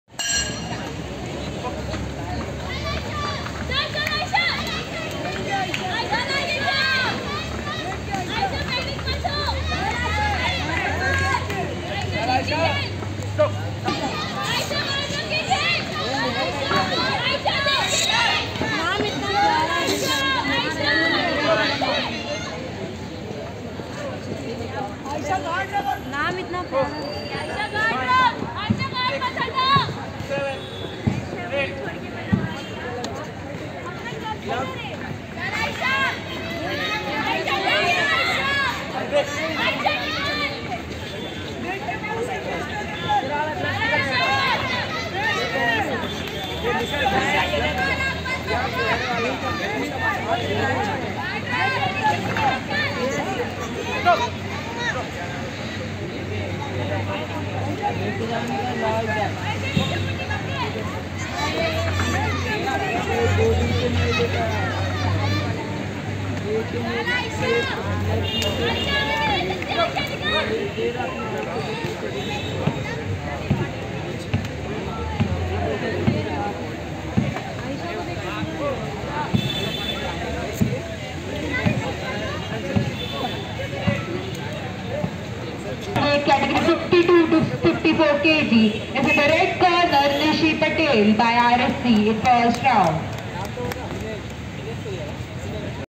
UTTAR MUMBAI KRIDA MAHOTSAV, POINSUR KHEL MAHOTSAV JUNIOR BOXING CHAMPIONSHIP 2023,KANDIVALI, MUMBAI
BOXING TOURNAMENT :
UTTAR MUMBAI KRIDA MAHOTSAV, POINSUR KHEL MAHOTSAV CUB CLASS / CADET / SUB JUNIOR (SCHOOL AGE) / JUNIOR / YOUTH / ELITE BOXING CHAMPIONSHIP 2023
Day & Date : 5th to 9th December 2023
VENUE: Poinsur Gymkhana, Kandivali West, Mumbai,  Maharashtra

The main aim of this video is to promote Boxing as Sports in  all over Mumbai, Maharashtra and India in Cities and Villages.